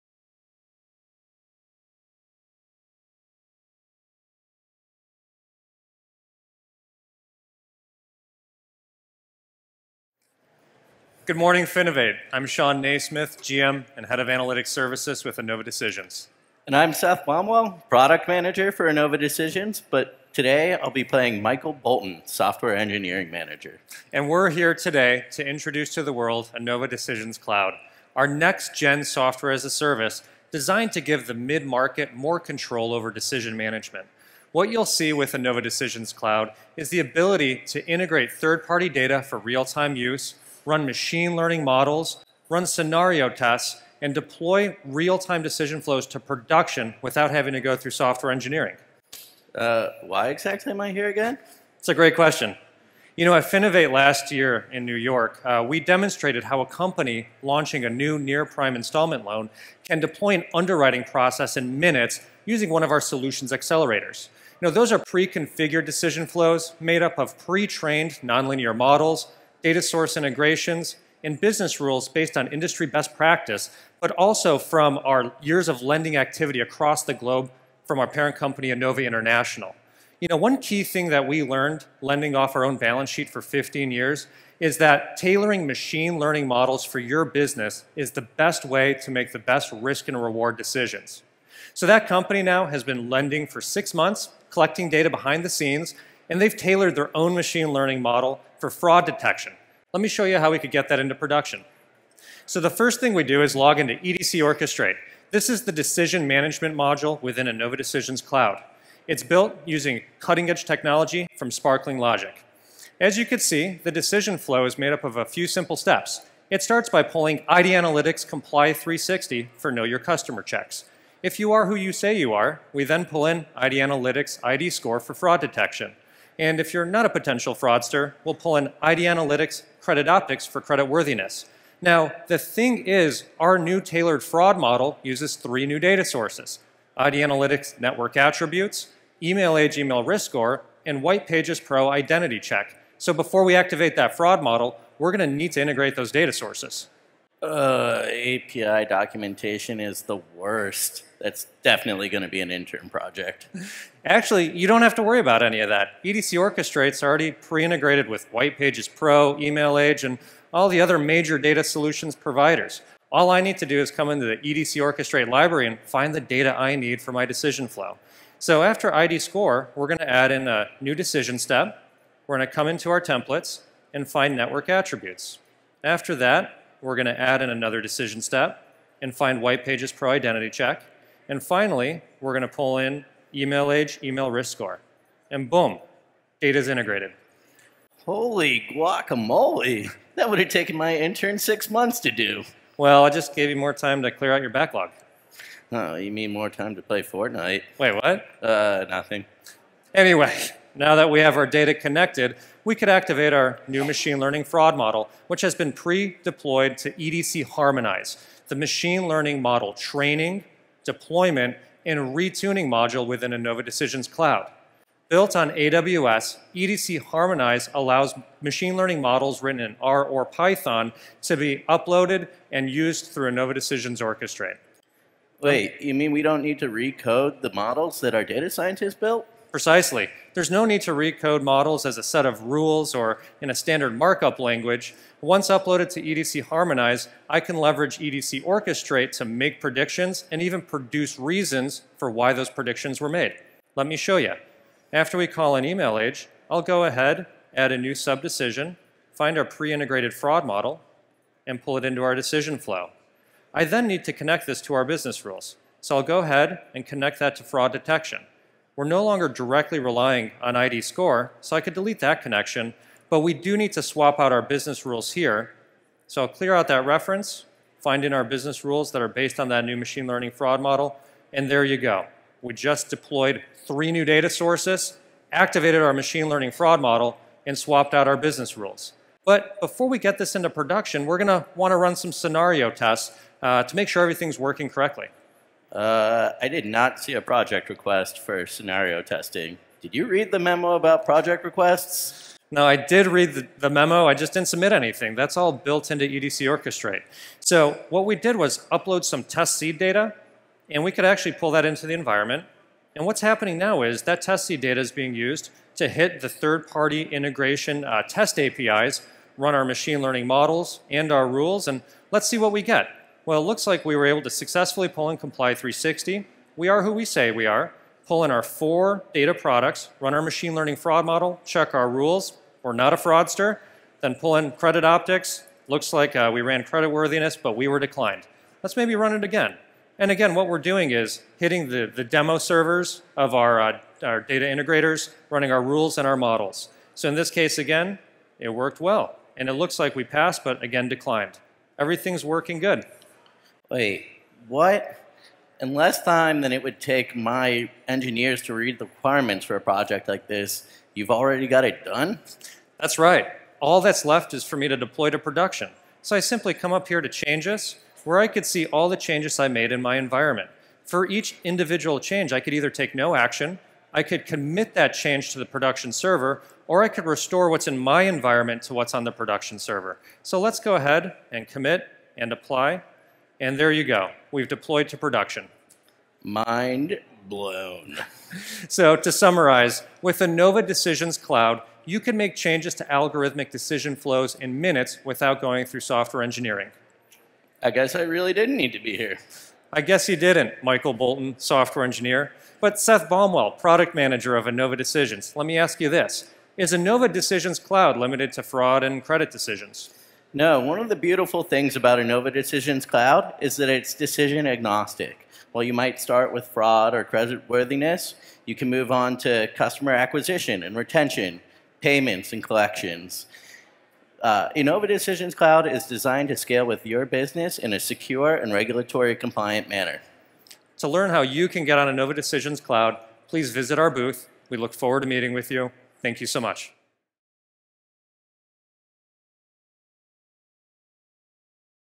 FinovateSpring 2019 / Enova Decisions
Enova Decisions demoed at FinovateSpring in 2019.

Actionable analytics is the backbone of NYSE-listed Enova International, a global online lending company. In the past 14 years, the analytics team has applied predictive and prescriptive analytics to fraud detection, credit risk management, and customer retention and built the Colossus™ Digital Decisioning Platform to automate and optimize many of Enova's operational decisions. As a result, Enova has extended over $20 billion in credit to over 5 million customers worldwide. Enova Decisions was launched in 2016 to help businesses in financial services, insurance, healthcare, telecommunications, and higher education achieve similar outcomes by leveraging the same analytics expertise and decisioning technology.